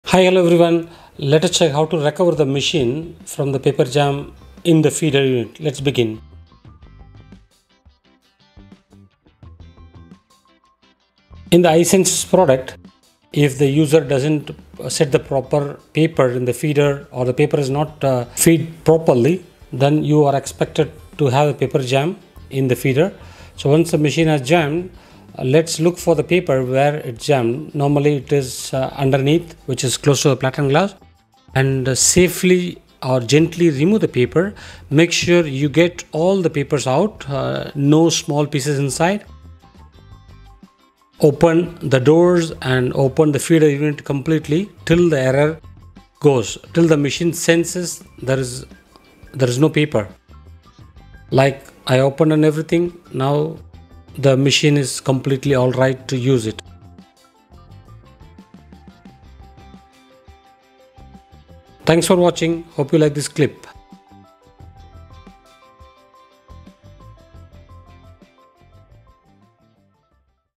iSENSYS MF 461 dw PAPER JAM IN FEEDER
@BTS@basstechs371

This video will help the user’s to recover a paper jam from iSENSYS MF-461dw . This issue can happen if the paper quality is not an appropriate and improper feeding. The jammed paper should be removed completely without any pieces on it. Please follow this clip and make use of it .